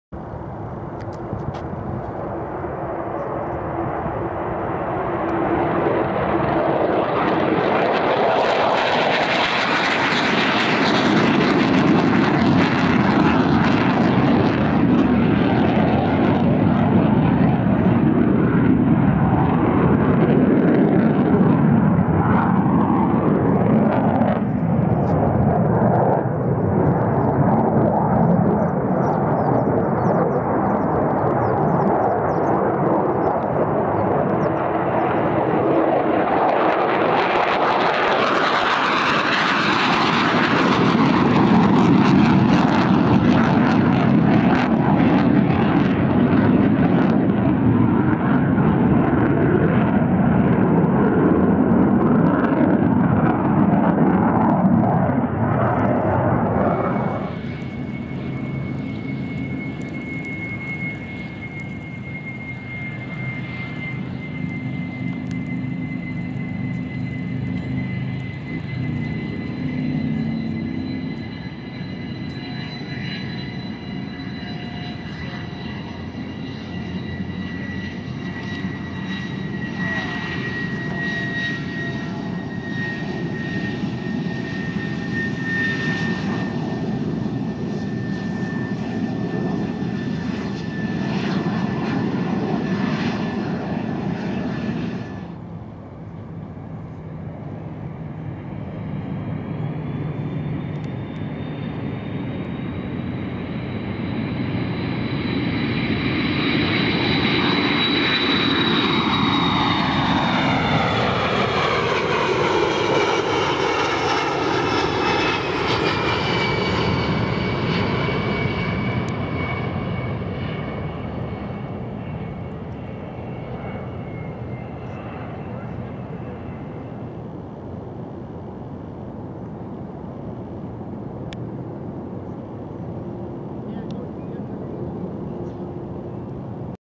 March ARB Activity
Went to the museum today, and since it was thursday, got to see some exercises :)